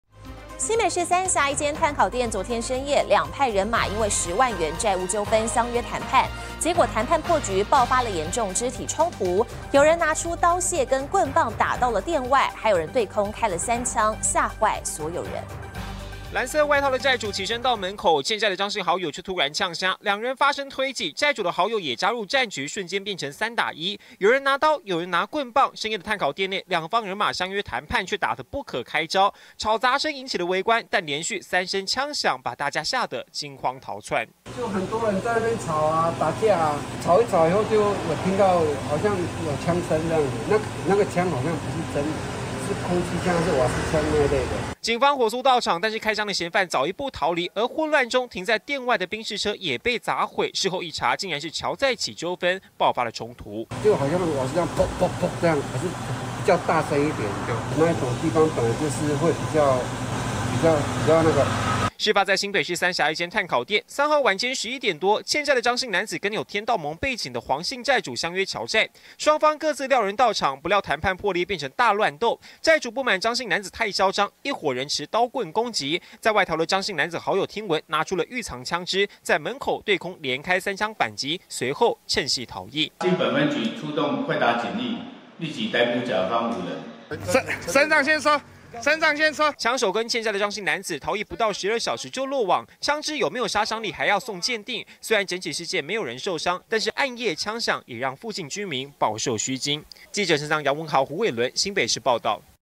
喬10萬債引爆大亂鬥 三峽炭烤店驚傳槍響
新北市三峽一間炭烤店，昨天(3日)深夜11點多，有兩派人馬因為10萬的債務糾紛相約談判，不料談判破局，爆發嚴重肢體衝突，有人拿出刀械跟棍棒打到店外，還有人對空開3槍，嚇壞所有人，幸好嫌犯全數落網。